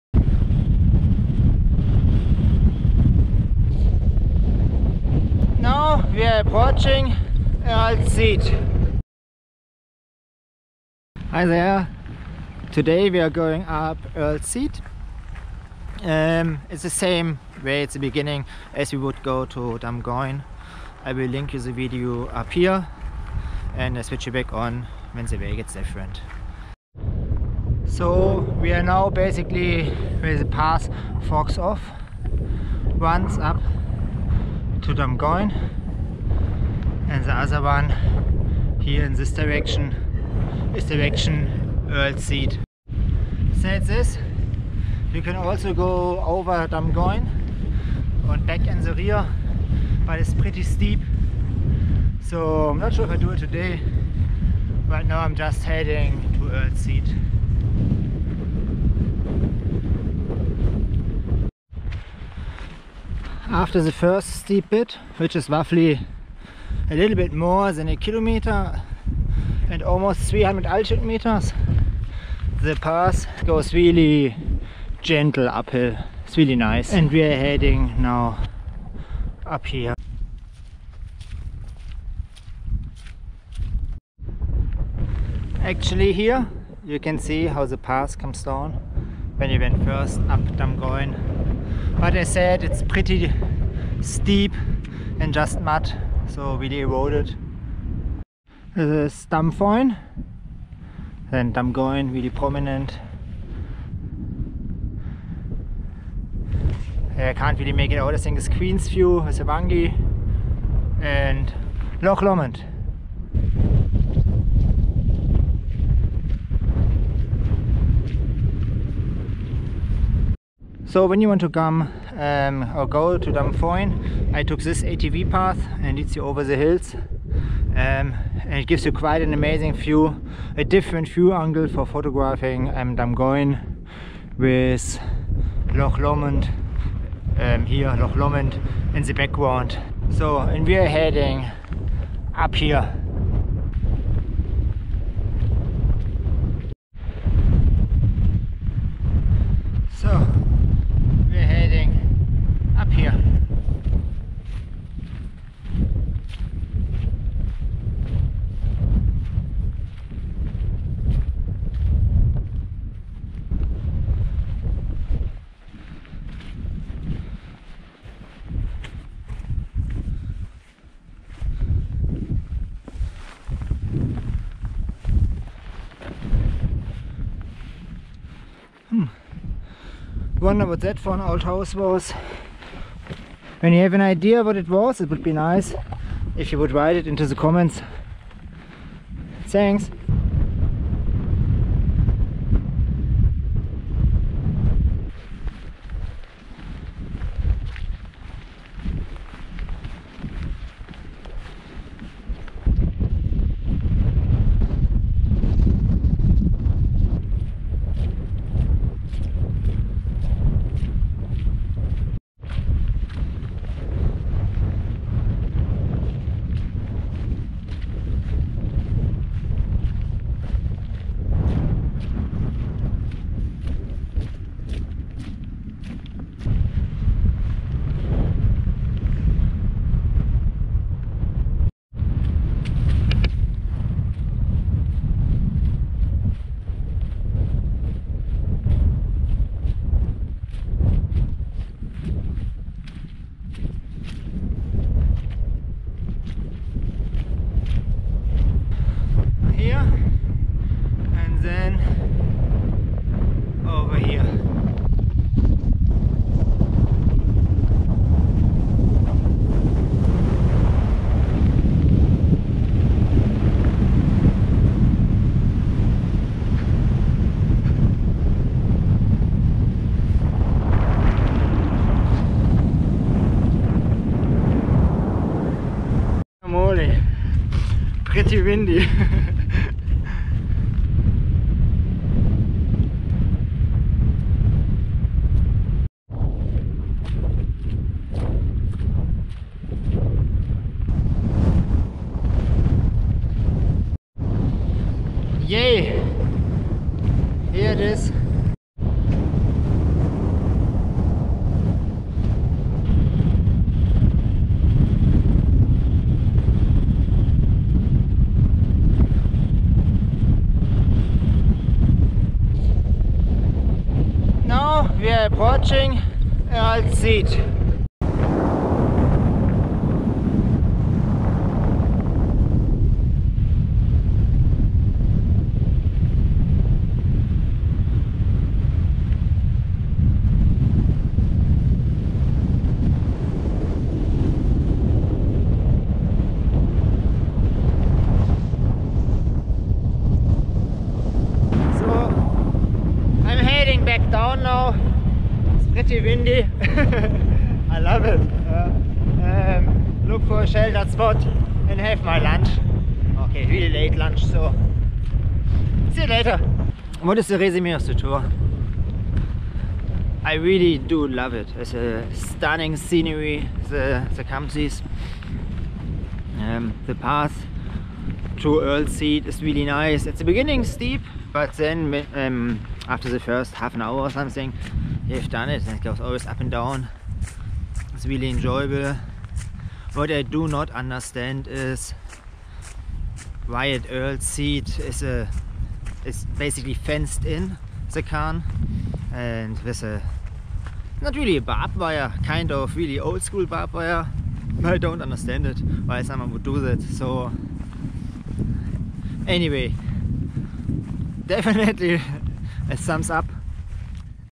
Hiking EARL’S SEAT in HIGH WINDS (Campsie Fells)
Today I went hiking in high winds, up Earl's Seat in the Campsie Fells (Scotland) just outside of Glasgow. Earl's Seat is the highest hill of the Campsie Fells in central Scotland with an elevation of 578m.

The hike for Earl's Seat starts just at the Glengoyne Distillery on A875 (there are roughly spaces for 5 cars). For the first 300 altitude meters or roughly 2.5 km the way to Earl's Seat and Dumgoyne is the same before the path for direction Earl's Seat forks off to the left. Here the path becomes more boggy but after a short while it becomes more solid again. Alternatively it is possible to walk over Dumgoyne and on the "rear" down - be beware at is quite steep and eroded!

After the first steep ascent the path goes gently up and every so often a bit down. The scenery becomes with every step more dramatic and well worth the effort. Boggy bits need to be passed so it is probably better to do that hike when there was a couple of days less/no rain.

To reach the summit of Earl's Seat you need to climb a kind of an old "barb-wire" fence which is around the cairn of Easrl's Seat.

Earl's Seat is a beautiful day hike in the campsie fells just outside of Glasgow and well worth the effort and by many undervalued!

According to my watch the hike is around 885 altitude meters.

Filmed entirely on the GoPro 7 Black.